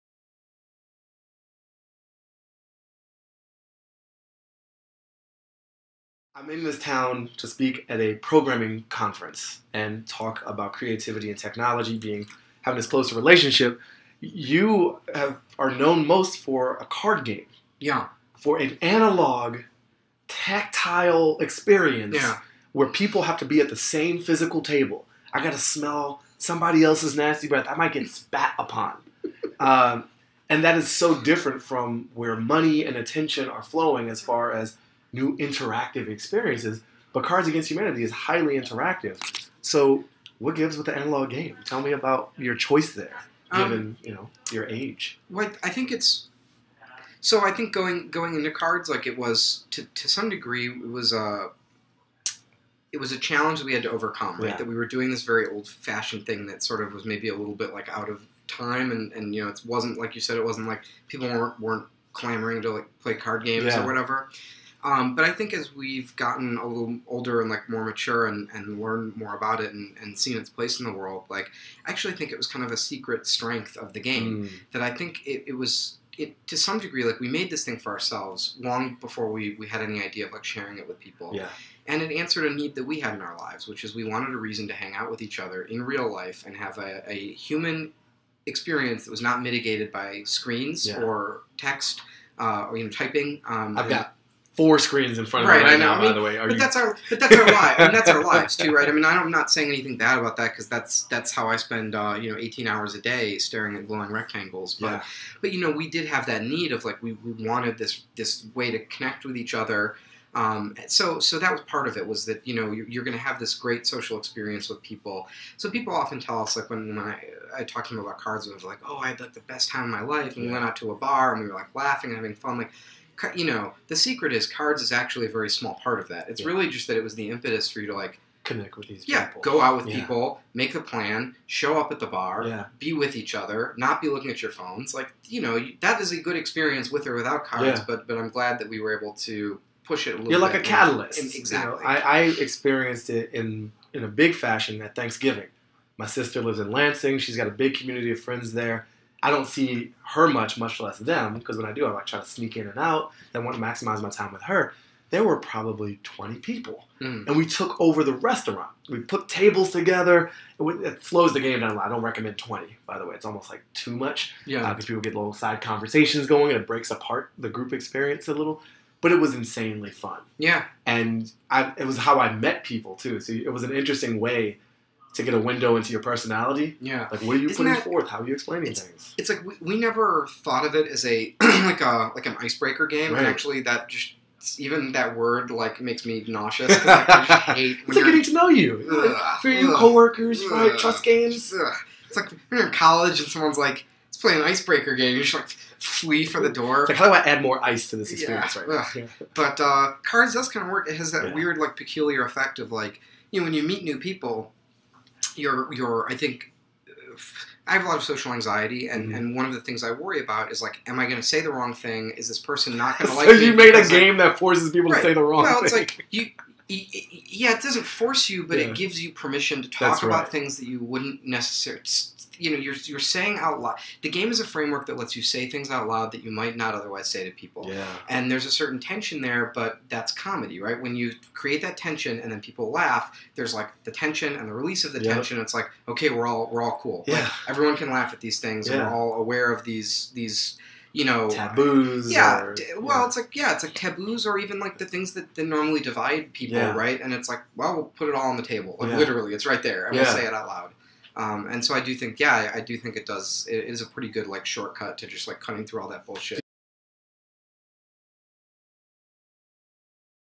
Talks With Interesting People - Max Temkin, Cards Against Humanity (Part 1) on Analog Games
Baratunde sat down with Max Temkin, the founder of Cards Against Humanity, April 24, 2014 in Chicago, Ill. to talk about important things of all sorts. Things like the relationship between comedy and technology, and how it all comes together in the delightfully offensive and even-more-delightfully analog Cards Against Humanity. This is part of their one hour conversation.